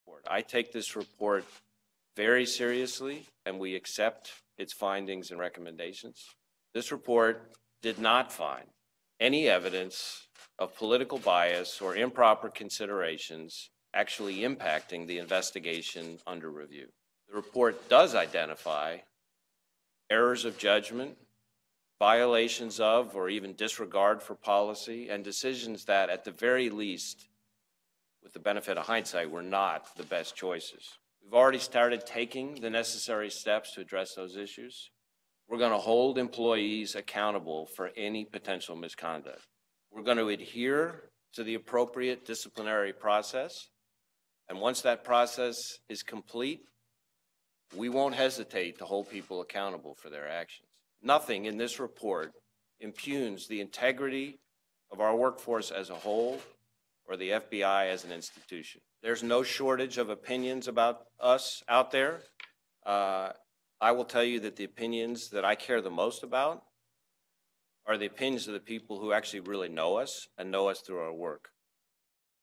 FBI: Report identifies disregard for policy, errors in judgement
An FBI lawyer exchanged messages after the election with another colleague and proclaimed: 'Vive la resistance.' The IG's new report on the FBI quotes an FBI I lawyer claiming to be 'numb' after the election. The lawyer wondered after the election 'what I could have done differently.' The lawyer worked on the Mueller probe until this year. The exchange was contained in a report blasted the FBI's handling of the Clinton email probe but said there was no 'bias.'